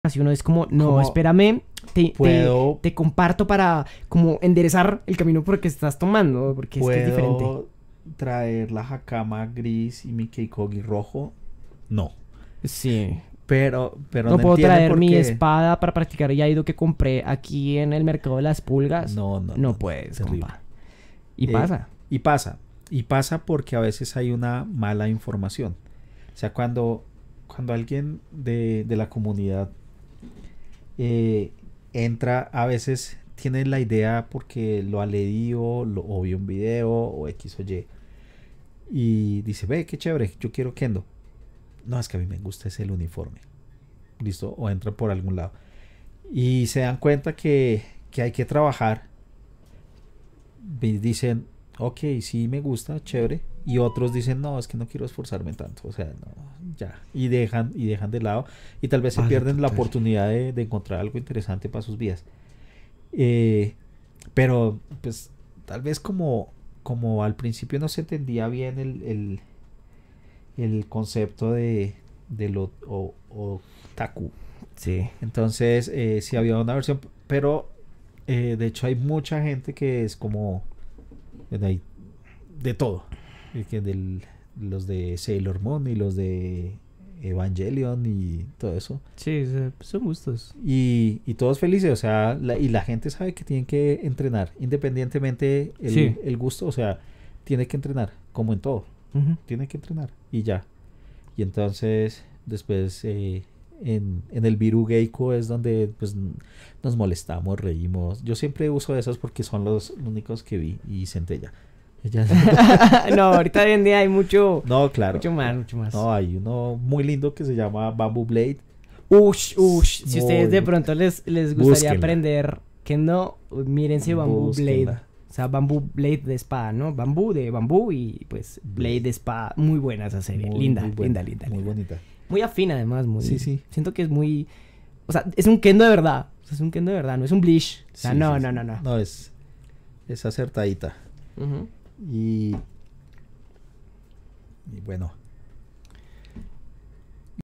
Los otaku en el Kendo
Esta semana conoceremos un poco del arte marcial del samurái, el Kendo , de la mano de Leonardo López.

Síguelo en Facebook: Dojo Akitsu KEndo Kai Iaido